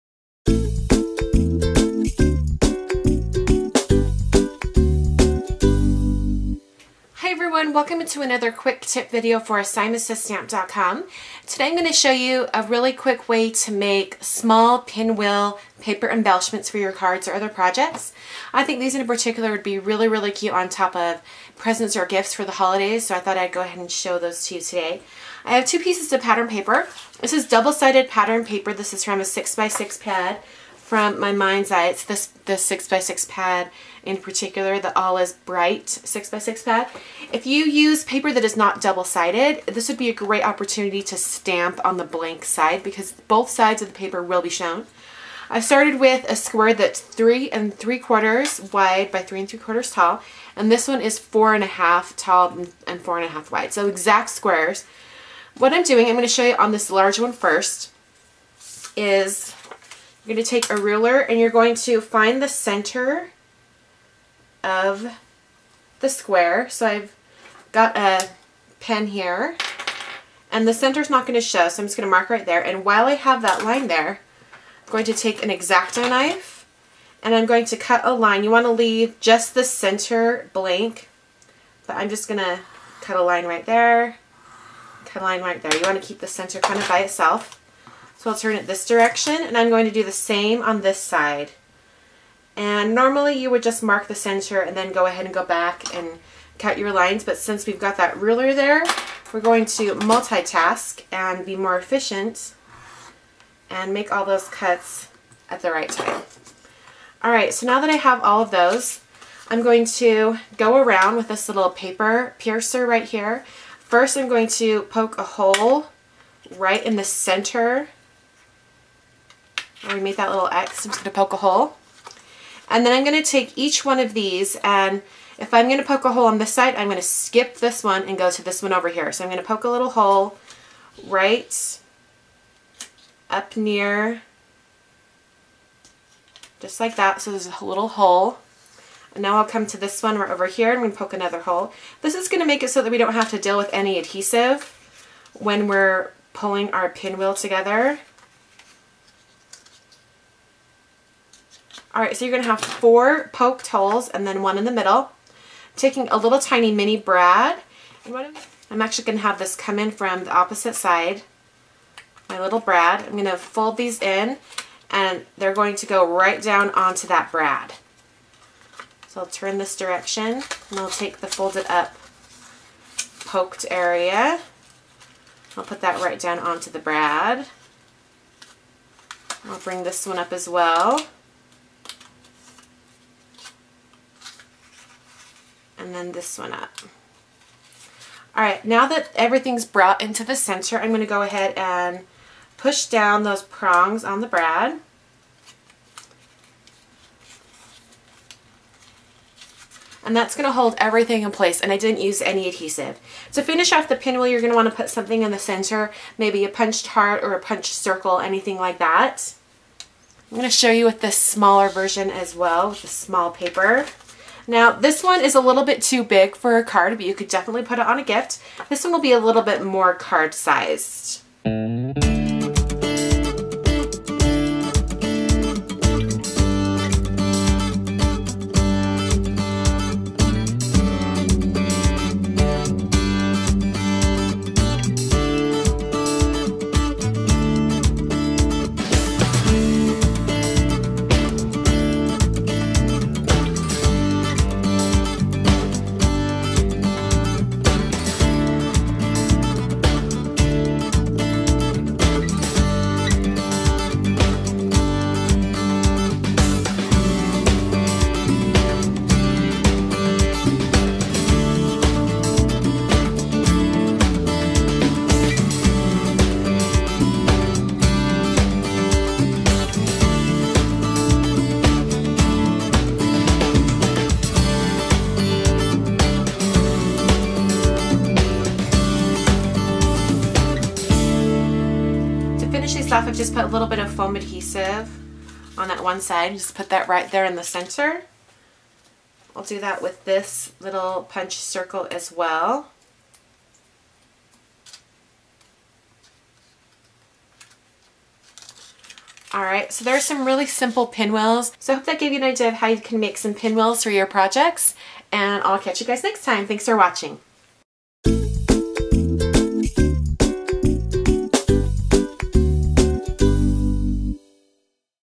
Quick Tip: Paper Pinwheels using NO ADHESIVE
How to create a paper pinwheel using no adhesive.

VINTAGE SIMON SAYS STAMP & SHOW CHALLENGE BLOG

SIMON SAYS STAMP CHALLENGE BLOG